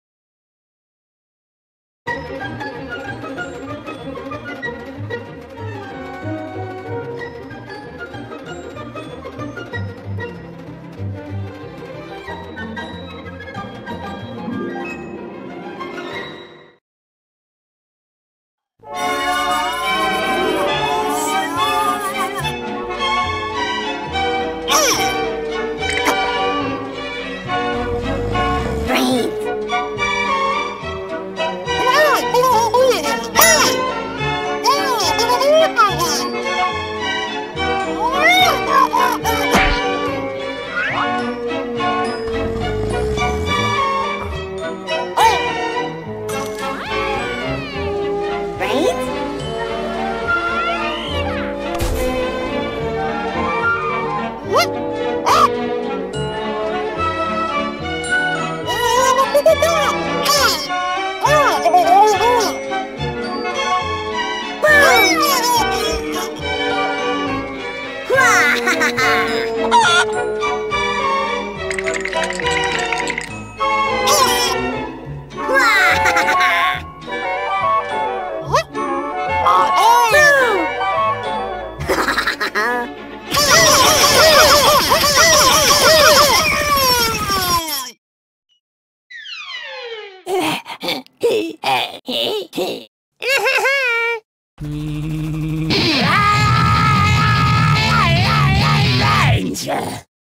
Imps In GW2
Since Plants vs Zombies Garden Warfare 2 is my most played game in life, i felt compelled to make a fan animation about it and here it is, a little criticism of annoying Imp players.